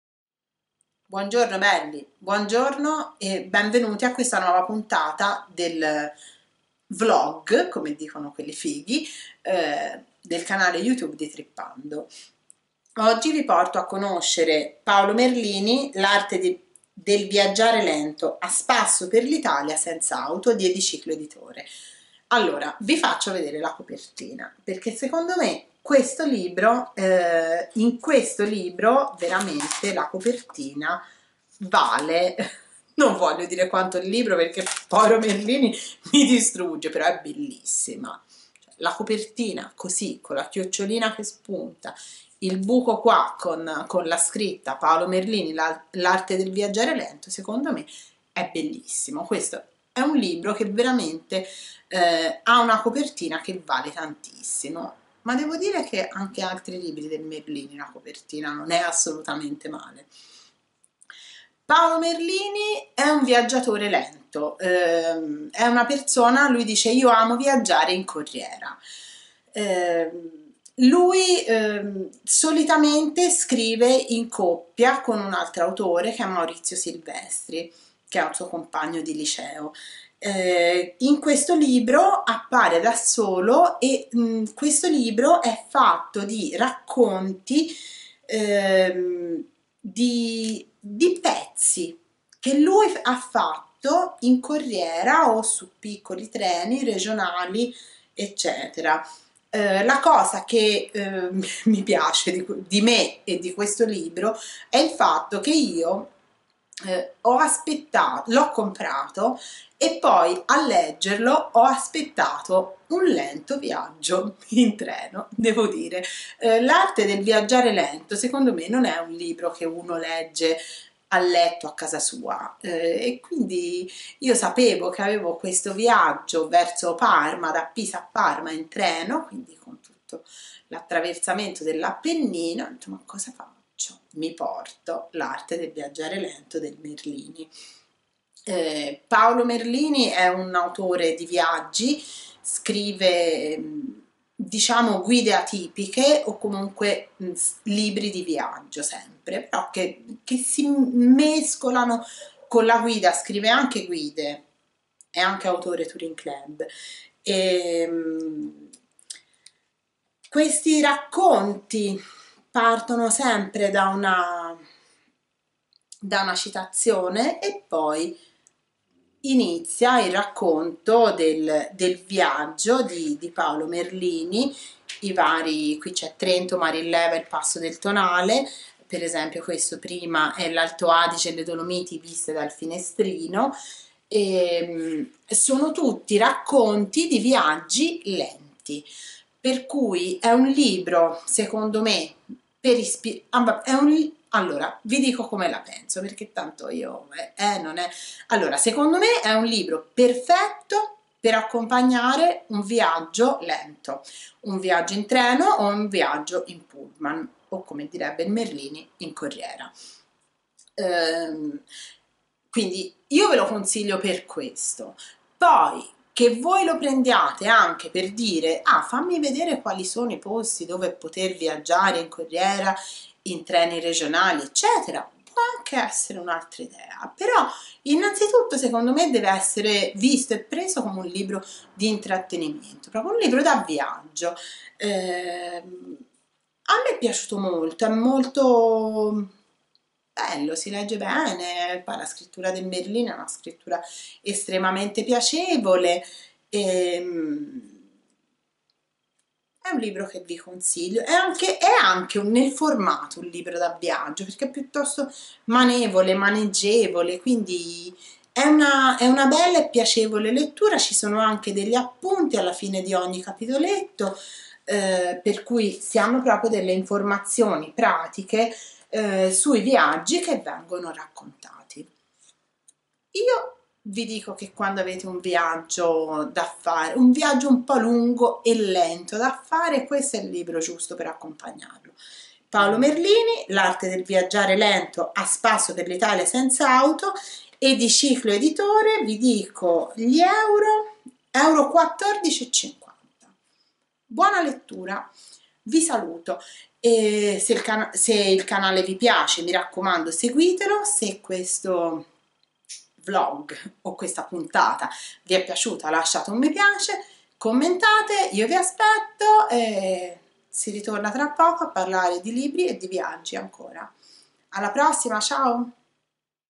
L'arte del viaggiare lento - Paolo Merlini - Italia
"L'arte del viaggiare lento", di Paolo Merlini, è un viaggio attraverso l'Italia a passo di lumaca, spostandosi in treno o in corriera. Si tratta di un libro di itinerari per lasciarsi ispirare alla ricerca del prossimo percorso lento, oppure un libro da leggere in viaggio, magari proprio durante un viaggio su un convoglio lento, come ho fatto io. Quelli di Merlini ne "L'arte del viaggiare lento" sono viaggi per perdersi in cui il viaggio non sta in una meta da raggiungere, bensì nel viaggio stesso, assaporato lentamente, preparato prima di partire, goduto in ogni istante.

La piccola lumaca in copertina spiega ed accoglie a pieno la filosofia dell'andare merliniano, fatto piccoli viaggi e grandi scoperte, di un andare cadenzato dalla lettura. Il viaggiare lento è un'arte, un saper tornare a tempi e ritmi antichi e un viaggiare fuori dalle masse, alla scoperta di luoghi seminascosti in cui scoprire cose belle e desuete, perché ogni luogo racchiude bellezze, basta saperle trovare!

L'arte del viaggiare lento
di Paolo Merlini
Ediciclo Editore, collana Ossigeno

Per acquisto diretto